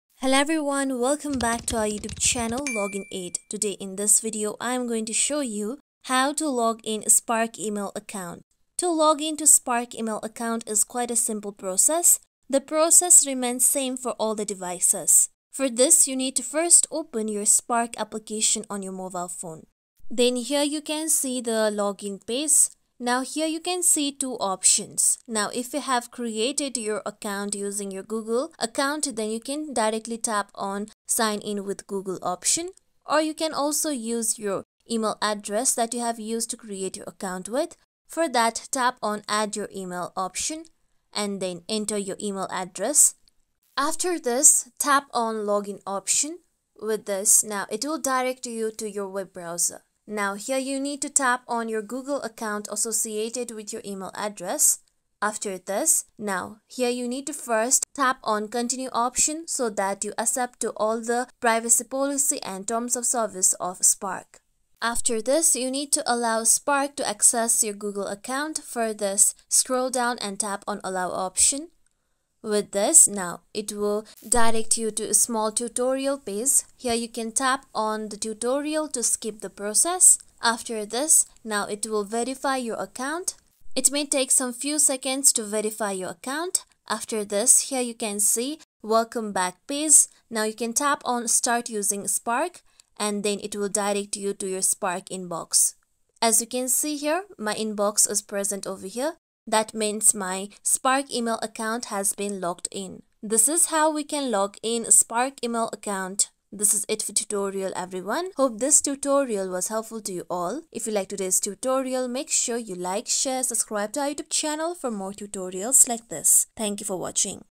How To Login Spark Email Account 2024 | Spark Sign In Tutorial | Spark App
Get step-by-step instructions on how to log in to your Spark email account. Whether you're new or just need a refresher, this guide simplifies the process. Gain quick access to your emails, manage your inbox, and make the most of Spark's powerful features by following this easy tutorial.